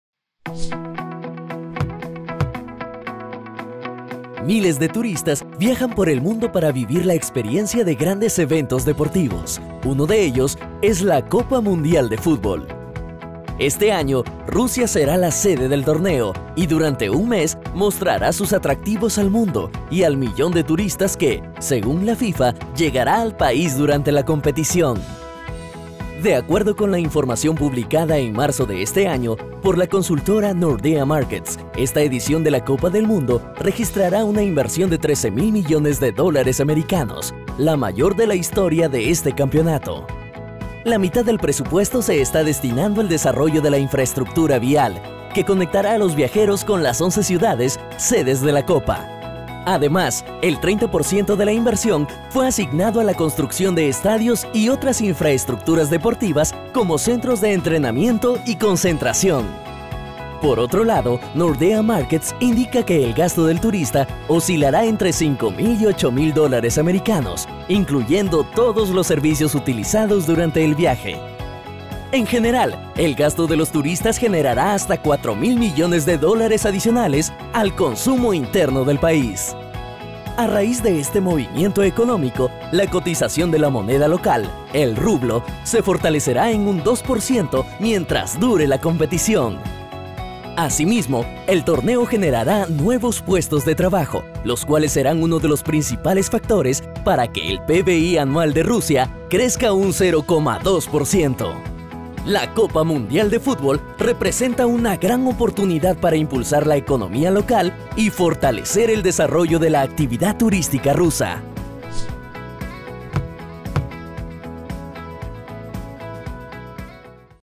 Economía y turismo: los campeones de Rusia 2018
TurismoIn | Según la FIFA, el Gobierno ruso espera recibir 1 millón de turistas que llegarán solo para vivir la experiencia del Mundial de fútbol, que se desarrollará en 11 ciudades del país del 14 de junio al 15 de julio, y se prevé por ello un impacto positivo en la economía local.